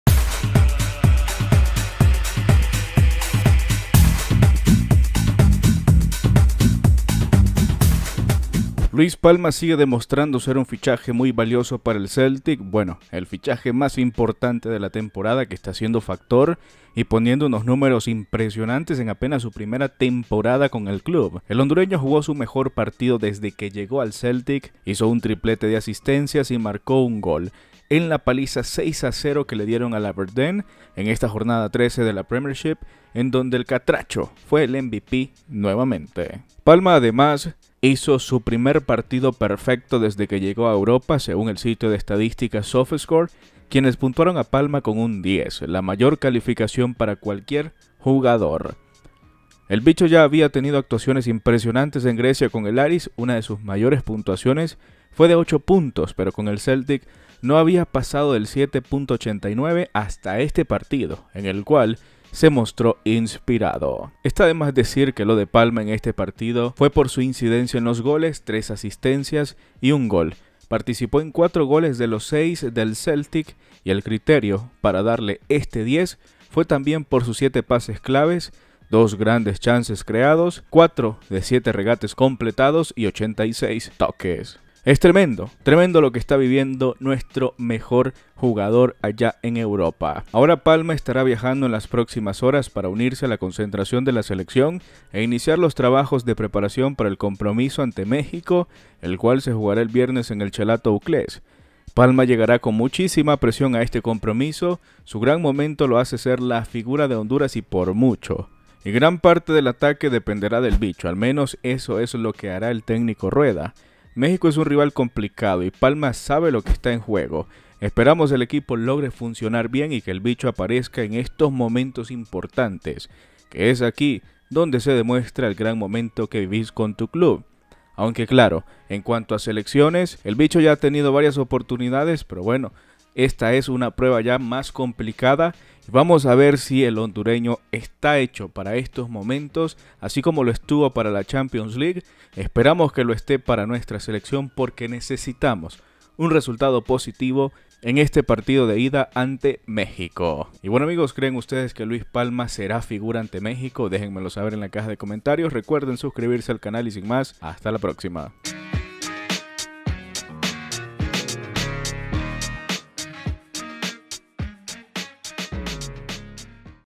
HONDUREÑO LUIS PALMA LOGRA SU PRIMER PARTIDO PERFECTO EN EUROPA🤩
Luis Palma logró lo que muy pocos han podido, conseguir la puntuación más alta en un partido, y el bicho ha entrado entre esos grandes jugadores que han sobresalido en Europa.